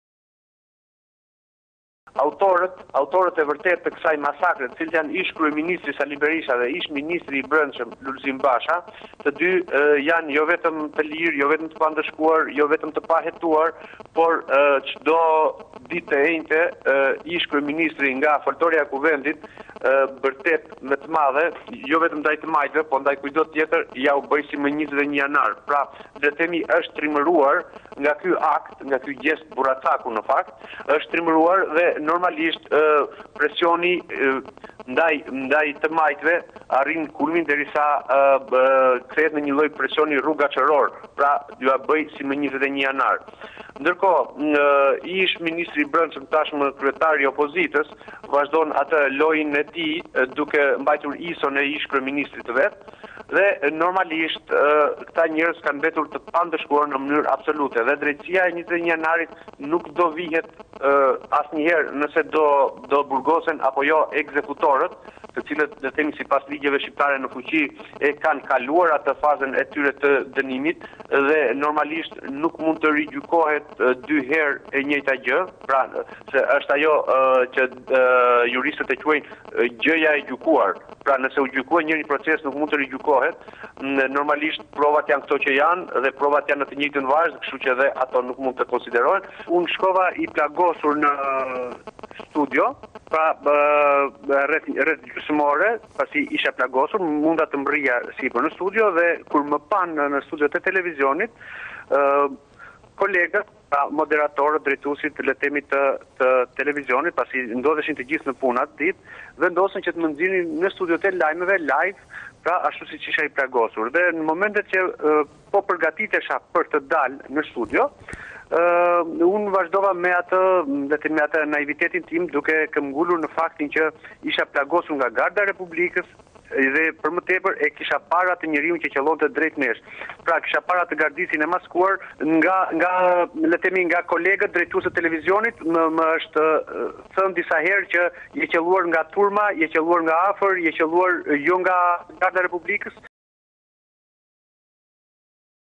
Report TV - Gazetari u plagos në 21 janar: Të dënohen vrasësit, Berisha-Basha
"Autorët e vërtetë Sali Berisha dhe Lulzim Basha janë jo vetëm të lirë e të pandëshkuar por edhe çdo të enjte Berisha bërtet 'ua bëj si me 21 Janar".  Kështu u shpreh sot në një lidhje telefonike për Report Tv,Fatos Mahmutaj, gazetari që u plagos me 21 janar te vitit 2011, në protesten e opozitës përballë kryeministrisë.

Mahmutaj rrëfen momentin e tmerrshëm sesi u vra Hekuran Deda, dhe si e kuptoi se ishte plagosur dhe vetë.

Gazetari theksoi drejtësia është kapur kokë e këmbë nga Berisha pasi gjyqtarët që dhanë vendimin për 21 janarin janë nomenklaturë e Argita Berishës, ku të gjithë janë emëruar përmes studios së saj.